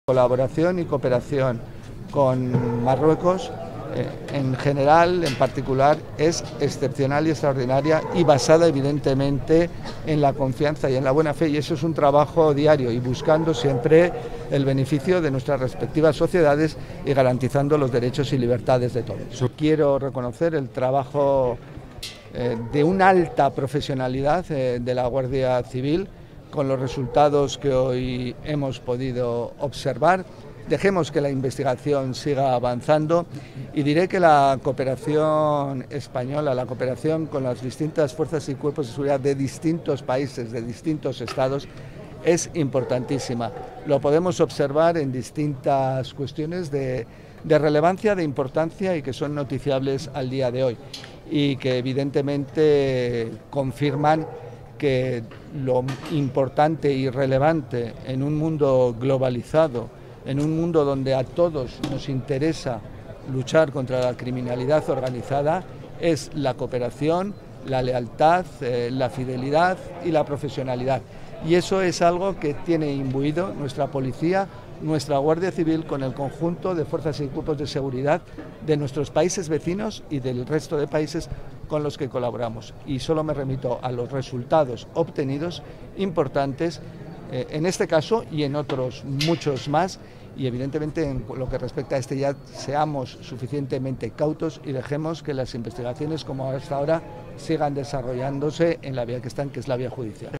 Sécurité. Le ministre espagnol de l'Intérieur parle de la coopération Maroc-Espagne
Le ministre espagnol de l’Intérieur, Fernando Grande-Marlaska, a qualifié, vendredi 20 septembre, d'"exceptionnelle et extraordinaire” la coopération entre le Maroc et l’Espagne en matière de sécurité.